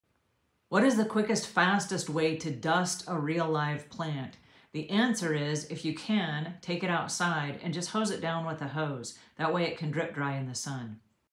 What is the Fastest Way to Dust a Real Plant? Pro Cleaning Tip
What is the fastest way to dust a real plant? If possible take the plant outside and put it on the patio or the deck. Rinse it with a garden hose and let it drip dry in the sun. This will remove the need for dusting indoors, and while the plants and plant stand are outdoors, it's a good opportunity to clean under the area where they reside indoors.

Classic Cylinder Plant Pot
Bib Apron - Unisex Black Aprons
Gardening Gloves
Powder-Free Nitrile Exam Gloves
How to Start Your Own House Cleaning Company